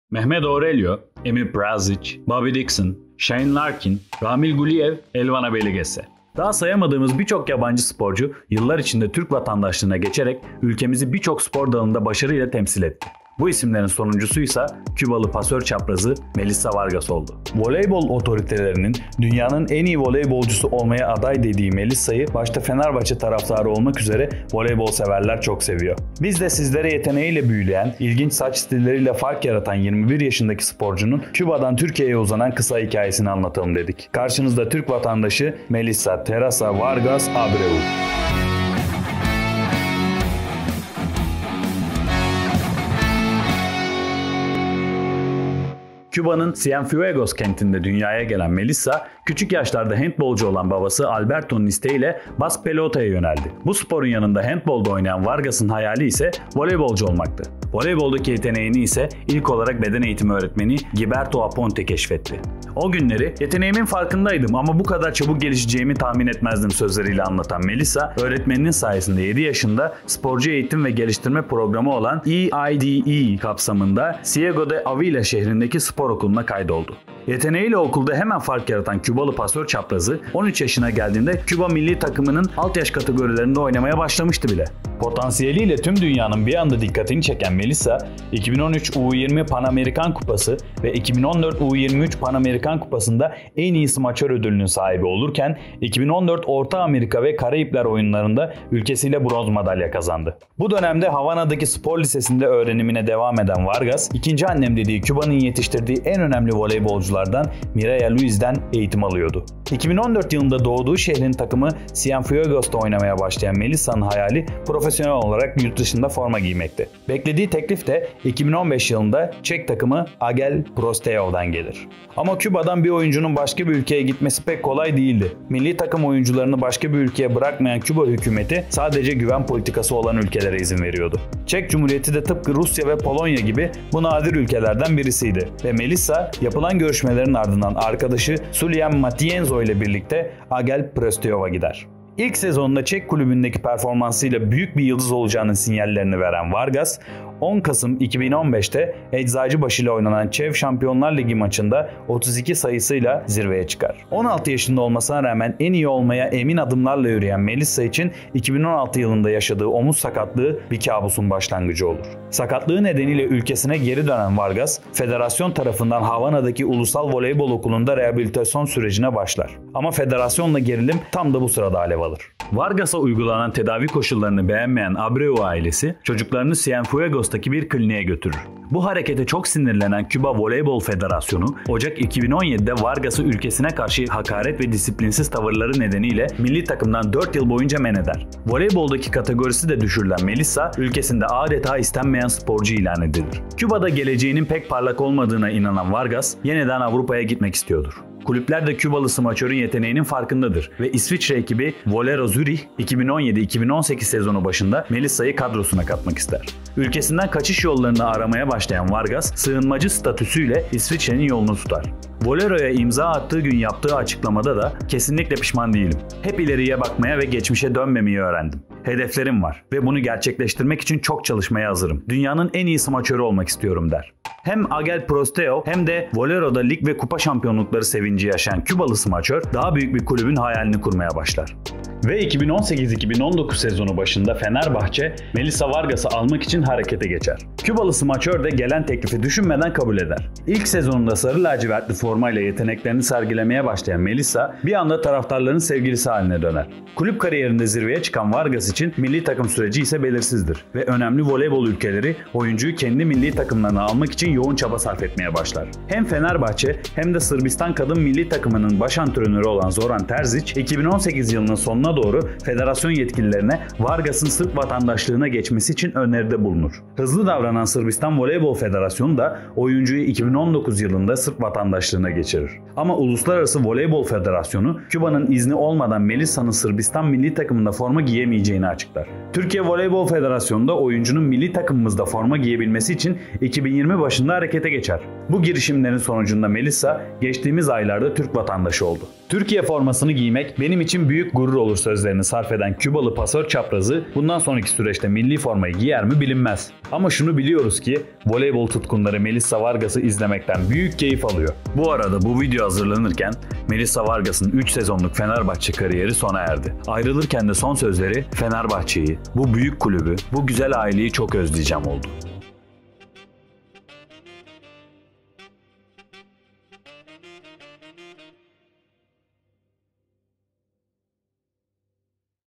Küba tarafından neden dört yıl men edildi? | Melissa Vargas
Mehmet Aurelio, Emir Preldzic, Bobby Dixon, Shane Larkin, Ramil Guliyev, Elvan Abeylegesse…

Daha sayamadığımız birçok yabancı sporcu, yıllar içinde Türk vatandaşlığına geçerek ülkemizi birçok spor dalında başarıyla temsil etti.

Bu isimlerin sonuncusu ise Kübalı pasör çaprazı Melissa Vargas oldu.

Voleybol otoritelerinin ''Dünyanın en iyi voleybolcusu olmaya aday'' dediği Melissa'yı başta Fenerbahçe taraftarı olmak üzere voleybol severler çok seviyor.

Biz de sizlere yeteneğiyle büyüleyen, ilginç saç stilleriyle fark yaratan 21 yaşındaki sporcunun Küba'dan Türkiye'ye uzanan kısa hikayesini anlatalım dedik.

Karşınızda Türk vatandaşı Melissa Teresa Vargas Abreu!!!